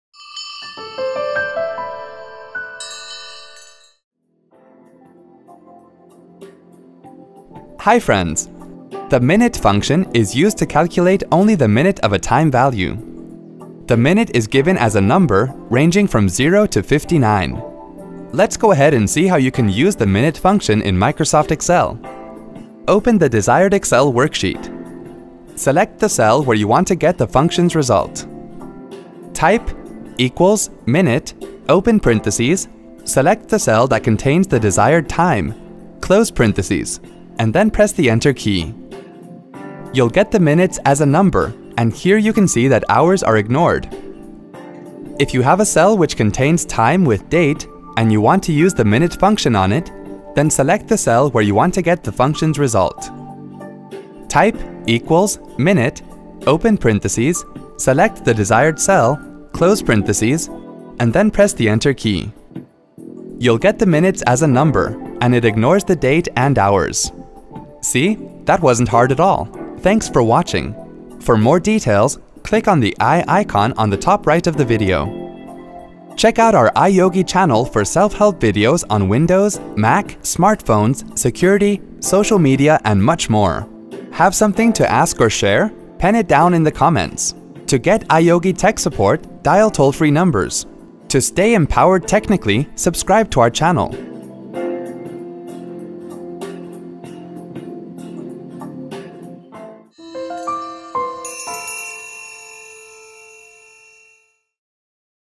How to use MINUTE function in Excel :Tutorial
The MINUTE function is used to calculate only the minute of a time value. The minute is given as a number, ranging from 0 to 59. Want to use MINUTE function in Microsoft® Excel? Watch this video and follow the steps to do so.